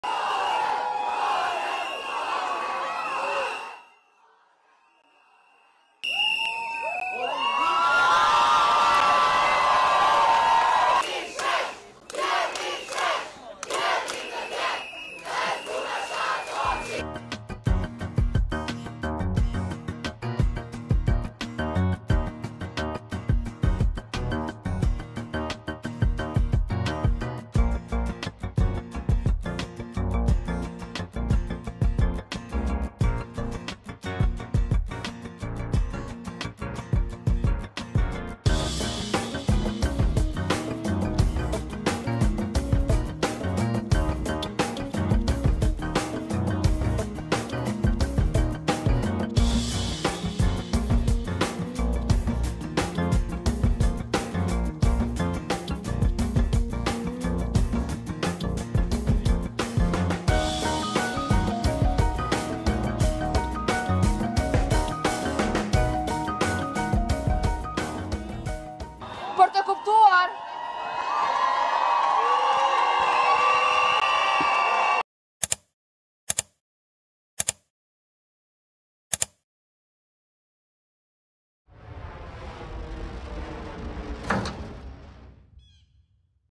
Curicu (11) u Prištini petorica silovala satima, tisuće na ulici traži smrtnu kaznu za njih
Jedan od osumnjičenih je 26-godišnji E.R., priveli su i njegovog maloljetnog brata i još trojicu maloljetnika. Kosovski šef policije zbog ovog slučaja podnio je ostavku.

Imate priču?